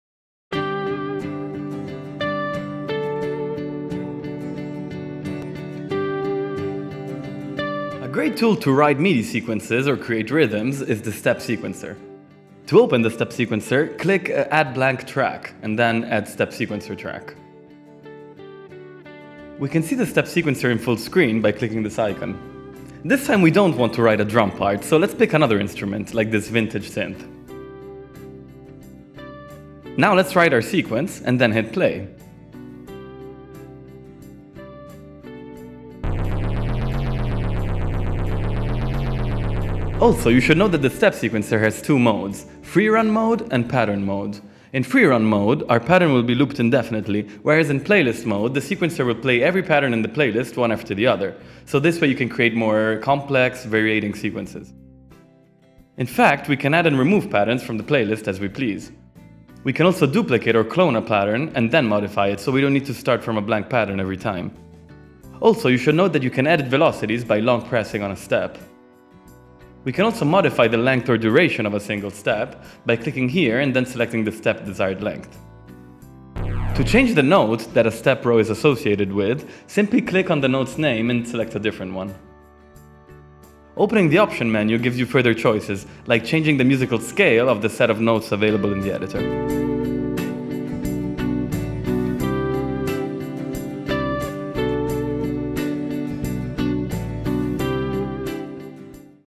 Step Sequencer Beat Maker | n-Track Studio iOS Tutorial Series (Intermediate)
In this tutorial, we have a look at using n-Track's mobile app built-in Step Sequencer to write melodies, beats or arpeggios.

n-Track Studio is a multitrack recording studio and digital audio workstation app for iOS, Android, Mac and PC.